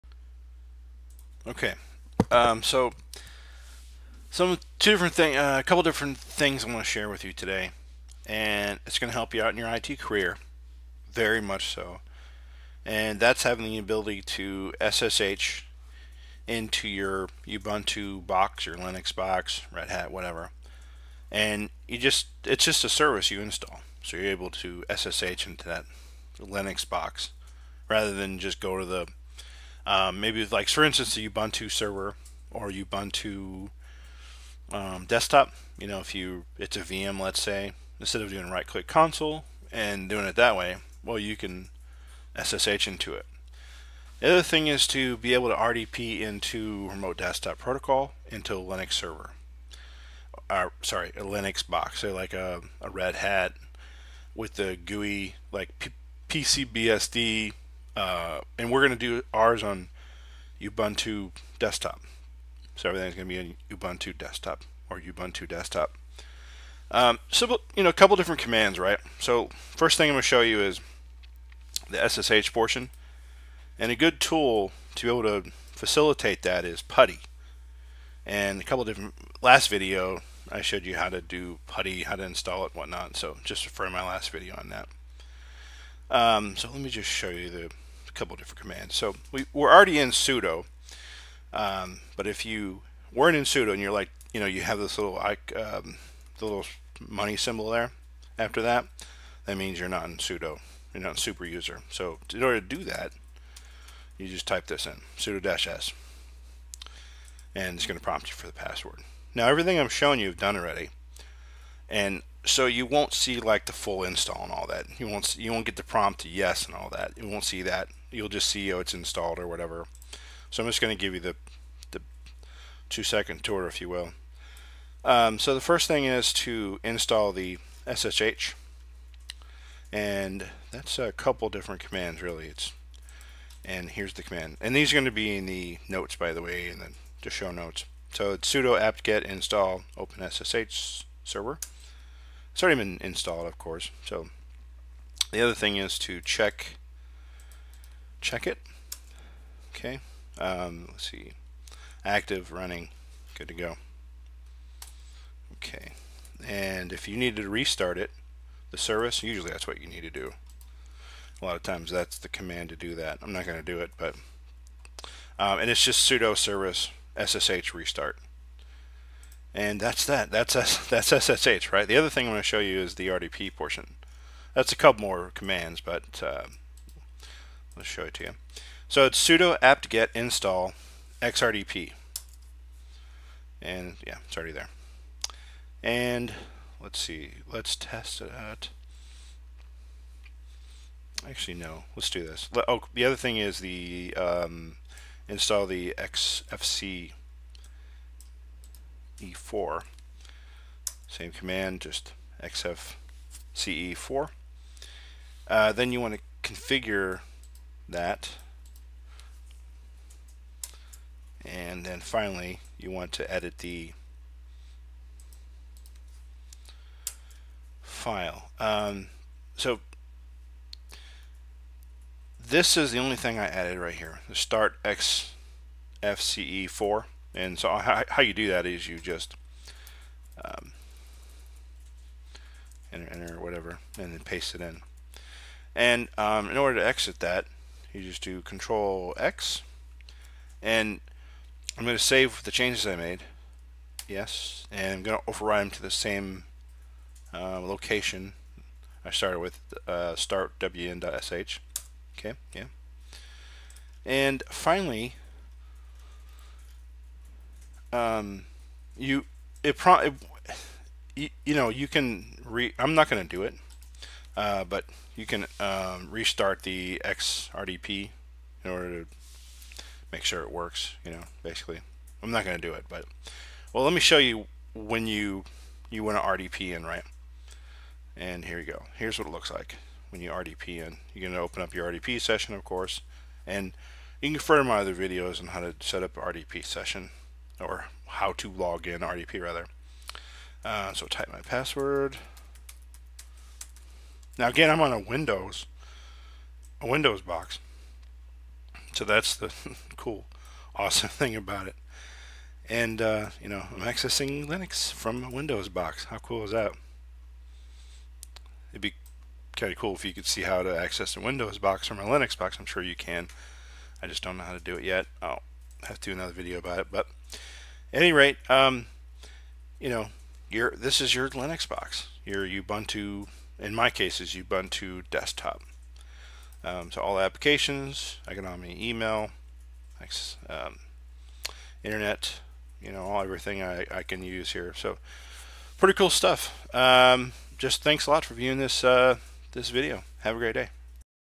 DIY installing SSH and RDP on a Ubuntu desktop (STEP BY STEP GUIDE)
How to install SSH and RDP on a Ubuntu desktop

Update your Ubuntu desktop    sudo apt-get update

Command to install SSH
sudo apt-get install openssh-server

sudo service ssh status

sudo nano /etc/ssh/sshd_config

then restart the ssh service

sudo service ssh restart

Commands to install xRDP

sudo apt-get install xrdp

sudo apt-get install xfce4

echo xfce4-session .xsession

( Angle brackets aren't allowed in your description. refer to video on exact command )

nano /etc/xrdp/startwm.sh

just type in startxfce4   and then ctrl X and Y to save changes

then restart the xrdp service

sudo service xrdp restart